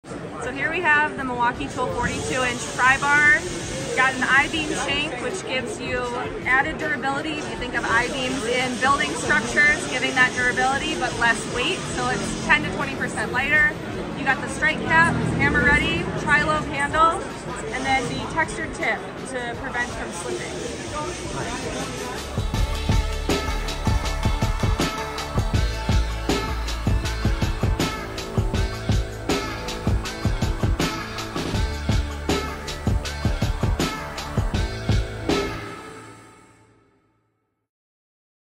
The New Milwaukee Tool 42 Inch Prybar: For Mechanics And Demolition Workers
Brian Eslick from How to Automotive show off new 42in Prybars from Milwaukee Tool from the Milwaukee Pipeline event.

New! How to Automotive T-shirts, Stickers and more click the